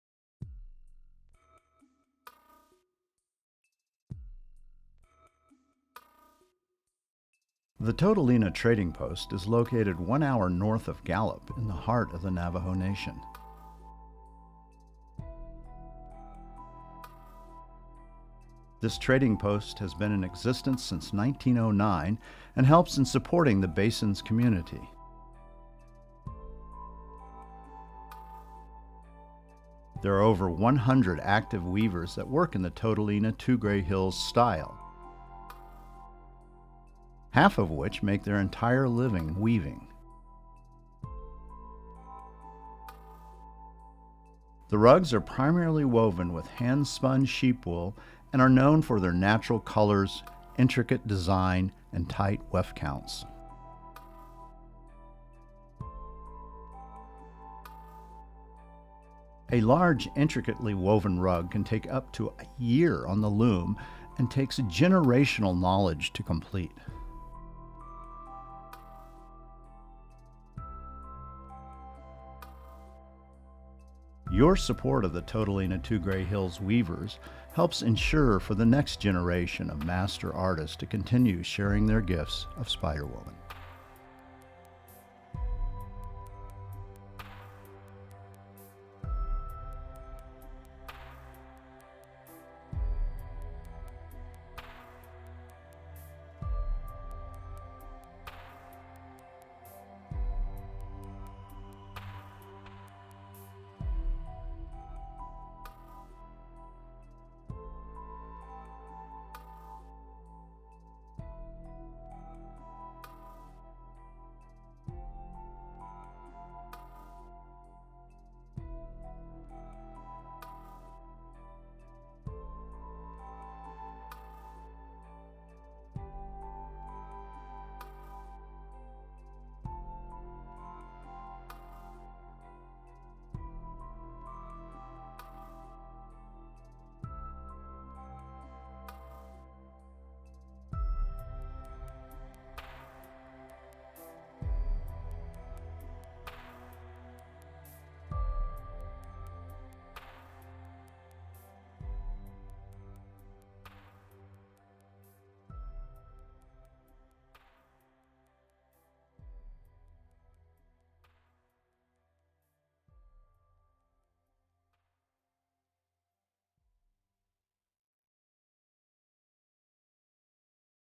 Contemporary Two Grey Hills Textile Collection | Navajo (Diné) Artist Insights
The Toadlena trading post is located 1 hour north of Gallup in the heart of the Navajo Nation. This trading post has been in existence since 1909 and helps in supporting the basin’s community.

There are over 100 active weavers that work in the Toadlena/Two Grey Hills style, half of which make their entire living from weaving. The rugs are primarily woven with hand spun sheep wool and are known for their natural colors, intricate design and tight weft counts. A large intricately woven rug can take up to a year on the loom and takes generational knowledge to complete

Your support of the Toadlena/Two Grey Hills weavers help insures for the next generation of master artist to continue sharing their gifts of Spider Woman.